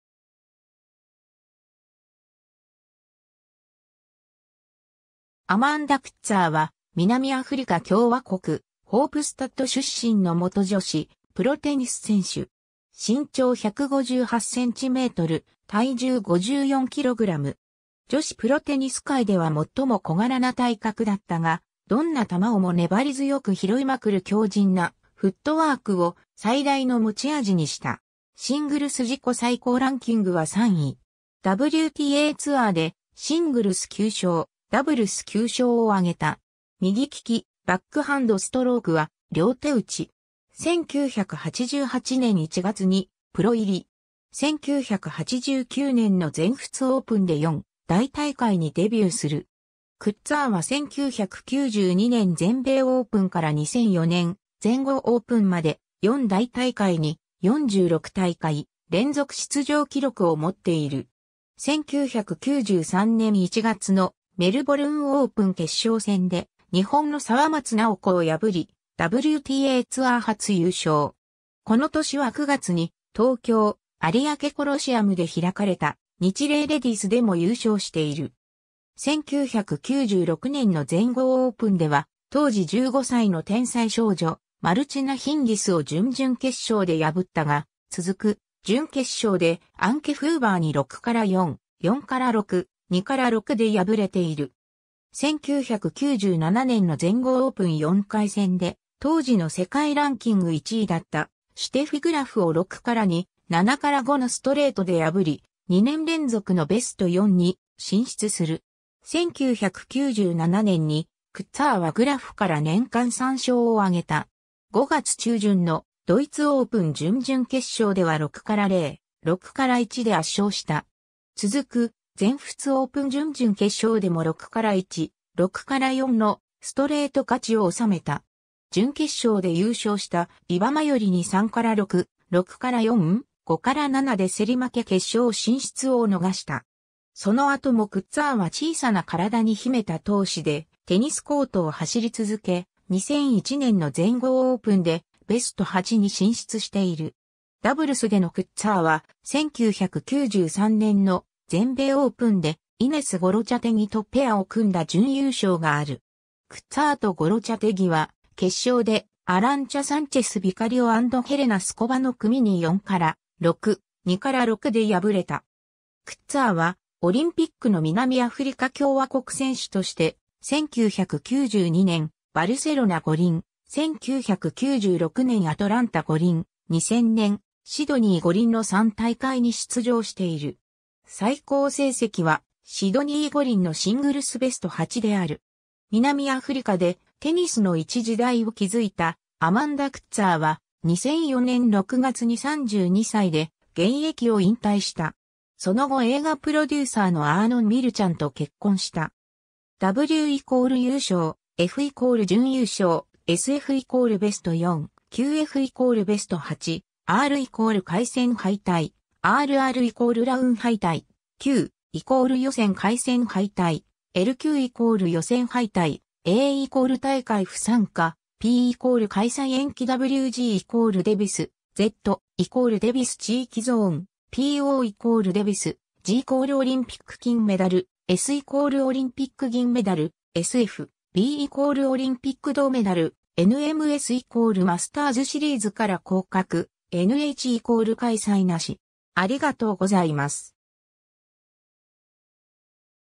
アマンダ・クッツァー
アマンダ・クッツァー（Amanda Coetzer, 1971年10月22日 - ）は、南アフリカ共和国・ホープスタッド出身の元女子プロテニス選手。
女子プロテニス界では最も小柄な体格だったが、どんな球をも粘り強く拾いまくる強靭なフットワークを最大の持ち味にした。
シングルス自己最高ランキングは3位。
WTAツアーでシングルス9勝、ダブルス9勝を挙げた。
 1988年1月にプロ入り。
1989年の全仏オープンで4大大会にデビューする。
クッツァーは1992年全米オープンから2004年全豪オープンまで、4大大会に「46大会」連続出場記録を持っている。
1993年1月の「メルボルン・オープン」決勝戦で日本の沢松奈生子を破り、WTAツアー初優勝。
この年は9月に東京・有明コロシアムで開かれた「ニチレイ・レディース」でも優勝している。
 1996年の全豪オープンでは、当時15歳の天才少女マルチナ・ヒンギスを準々決勝で破ったが、続く準決勝でアンケ・フーバーに 6-4, 4-6, 2-6 で敗れている。
1997年の全豪オープン4回戦で、当時の世界ランキング1位だったシュテフィ・グラフを 6-2, 7-5 のストレートで破り、2年連続のベスト4に進出する。
1997年に、クッツァーはグラフから年間3勝を挙げた。
5月中旬の「ドイツ・オープン」準々決勝では 6-0, 6-1 で圧勝した。
続く全仏オープン準々決勝でも 6-1, 6-4 のストレート勝ちを収めた。
準決勝で優勝したイバ・マヨリに 3-6, 6-4, 5-7 で競り負け決勝進出を逃した。
 その後もクッツァーは小さな身体に秘めた闘志でテニス・コートを走り続け、2001年の全豪オープンでベスト8に進出している（ビーナス・ウィリアムズに敗退）。
 ダブルスでのクッツァーは、1993年の全米オープンでイネス・ゴロチャテギ（アルゼンチン）とペアを組んだ準優勝がある。
クッツァーとゴロチャテギは、決勝でアランチャ・サンチェス・ビカリオ&ヘレナ・スコバの組に 4-6, 2-6 で敗れた。
 クッツァーはオリンピックの南アフリカ共和国選手として、1992年バルセロナ五輪、1996年アトランタ五輪、2000年シドニー五輪の3大会に出場している。
最高成績はシドニー五輪のシングルスベスト8である。
 南アフリカでテニスの一時代を築いたアマンダ・クッツァーは、2004年6月に32歳で現役を引退した。
その後映画プロデューサーのアーノン・ミルチャンと結婚した。